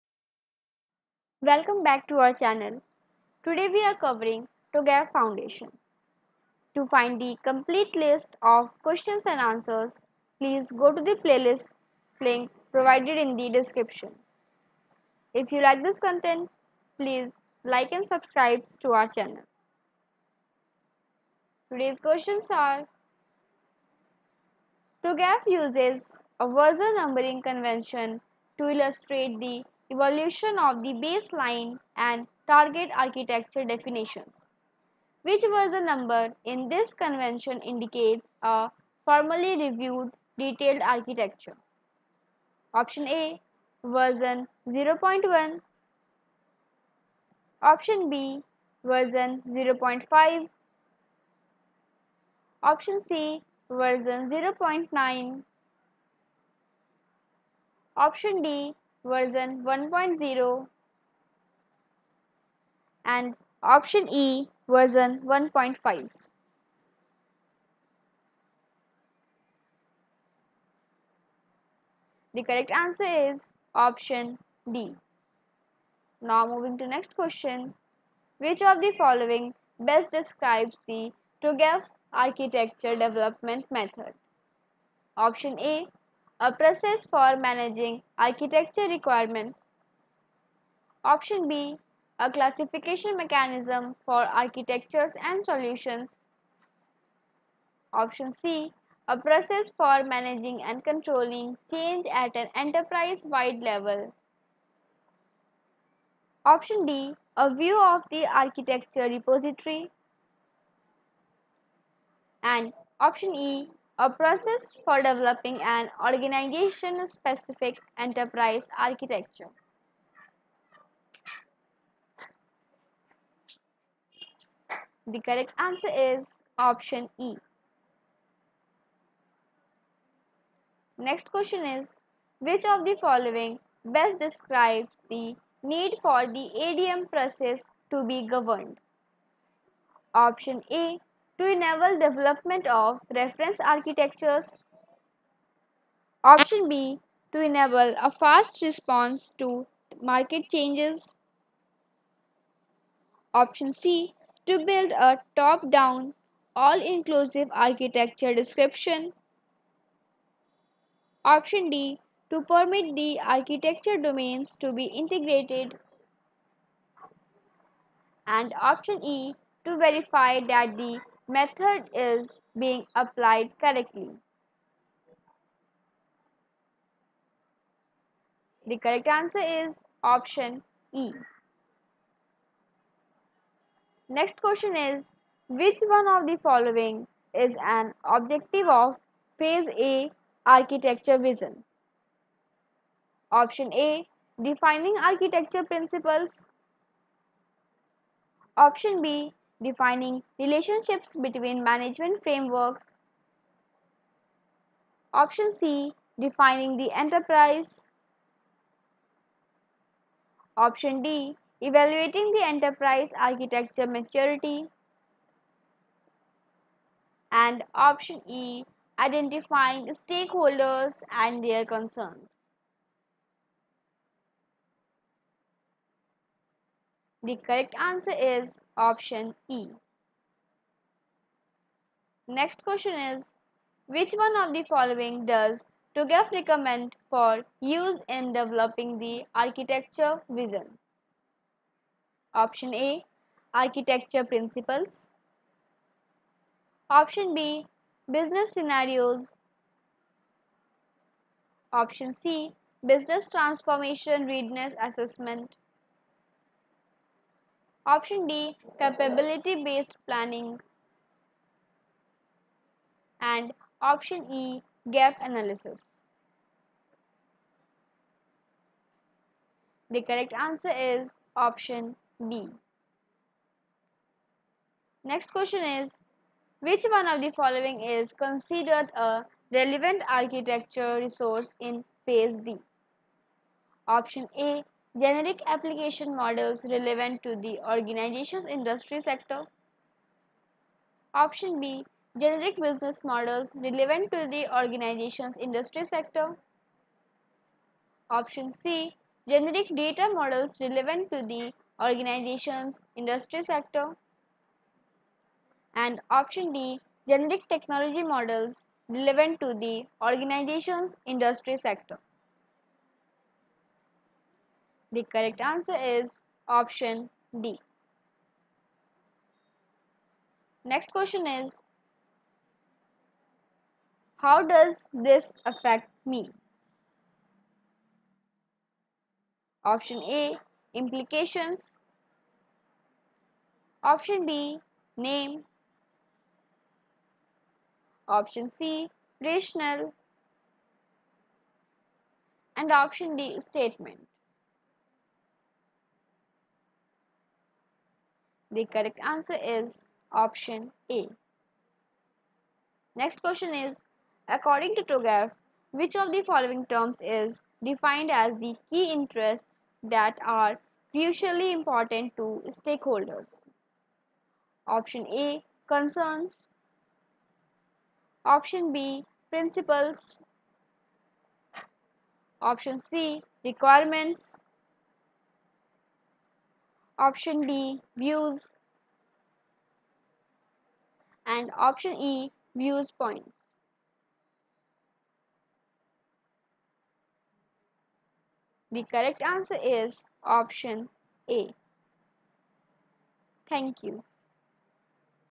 Part#10 - TOGAF Foundation (Level 1) -Questions and Answers
The TOGAF Foundation (Level 1) certification is a fundamental credential in the field of enterprise architecture offered by The Open Group Architecture Framework (TOGAF). This certification focuses on imparting foundational knowledge and understanding of the TOGAF framework, principles, and core concepts.

The certification covers key areas such as the TOGAF Architecture Development Method (ADM), architecture governance, architectural artifacts, and the role of enterprise architecture in business transformation. Candidates learn about the importance of aligning IT strategies with business goals, creating architectural models, and ensuring architectural governance and compliance.

By achieving the TOGAF Foundation (Level 1) certification, professionals demonstrate their proficiency in applying TOGAF principles to develop and manage enterprise architectures effectively. They gain a solid understanding of the terminology, structure, and concepts of the TOGAF framework.

This certification serves as a solid foundation for further progression in the field of enterprise architecture and positions professionals as skilled practitioners capable of architecting and managing complex IT landscapes. It enables them to contribute to strategic decision-making, drive organizational transformation, and ensure the alignment of IT initiatives with business objectives. access and attacks.